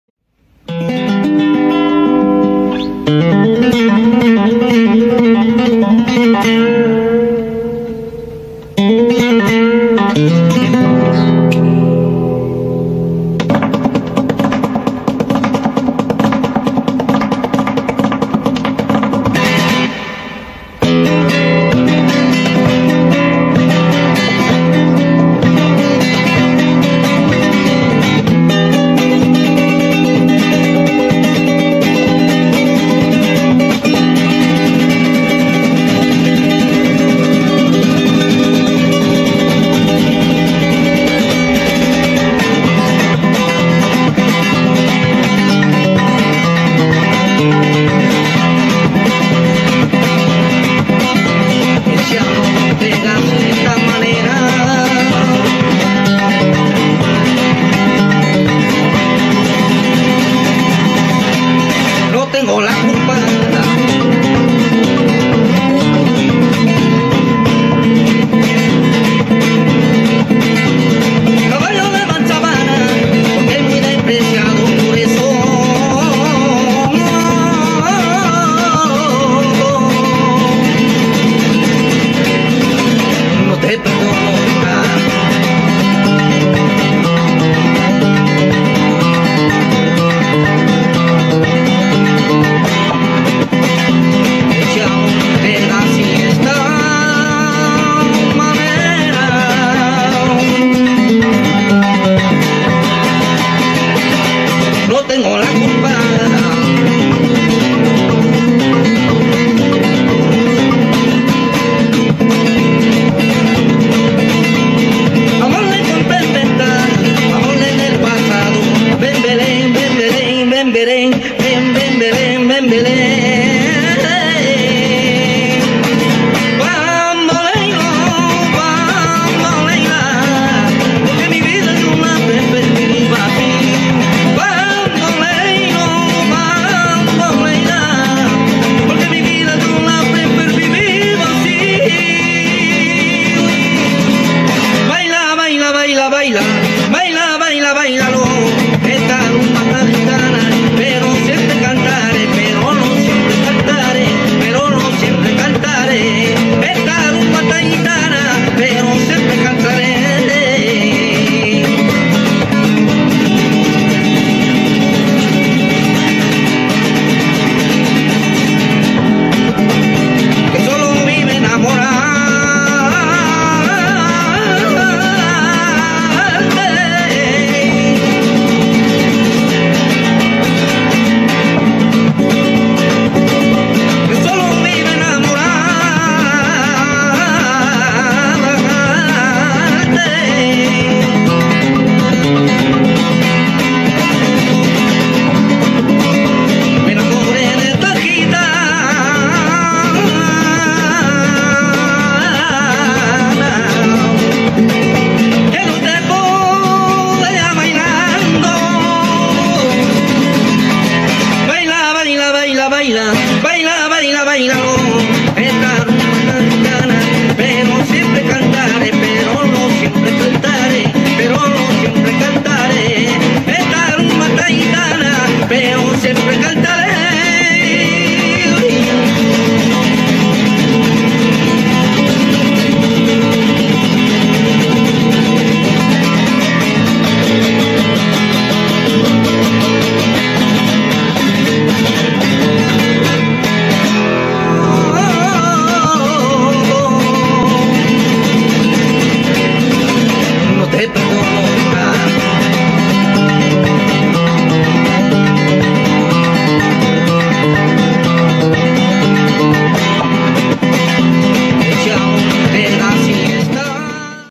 Gipsy Kings Medley ( Live )
Jonathan Arenas El Yoni casse littéralement la baraque en reprenant ce  Medley des Gipsy King !
Jonathan Arenas El Yoni literally breaks the house by taking over this Medley from the Gipsy King!

Captation vidéo : Jonathan Arenas el Yoni pour MCC
Monteur vidéo : Obbrac (MCC Team)